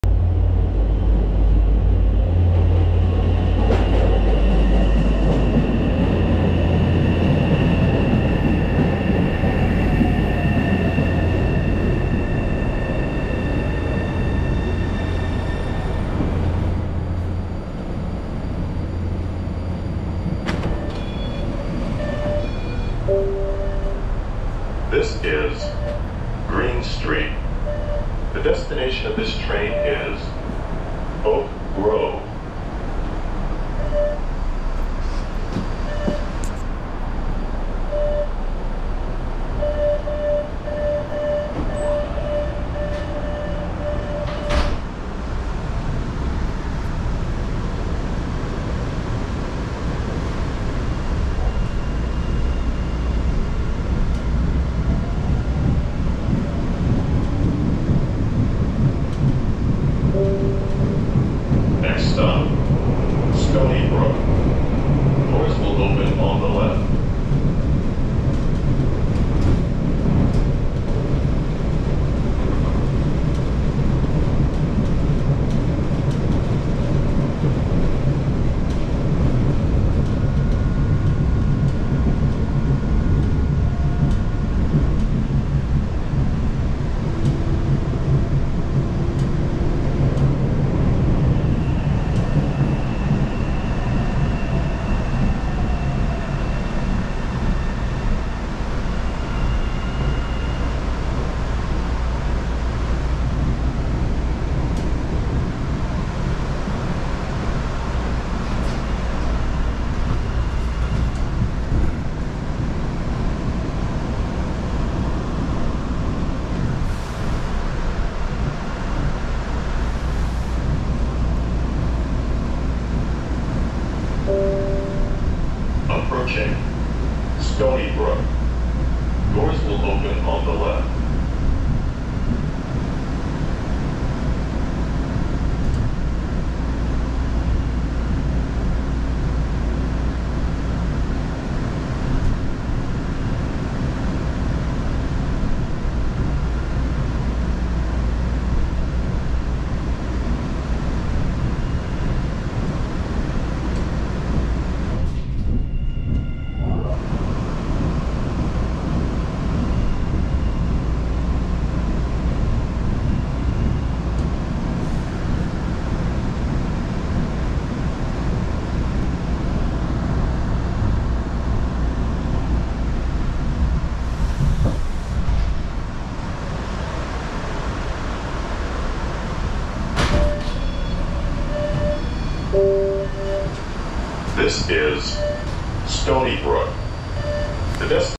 🚨 Is Boston Subway Clean & Safe? Ride Along!😮
Want to know if the Boston subway is clean and safe? Take a ride along with us as we explore the public transportation system in Boston. Get insider tips on riding the T and navigating the city like a pro!

This video shows you a ride on the Orange Line T in Boston, Massachusetts.

🌎Collect Bitcoin Rewards Walking Around Boston - sMiles App🌎

Orange Line T MBTA
Green St
Stony Brook
Are there any slow sections where the T is barely moving?  Should they fix these things once and for all?  How clean is it?  Is it safe?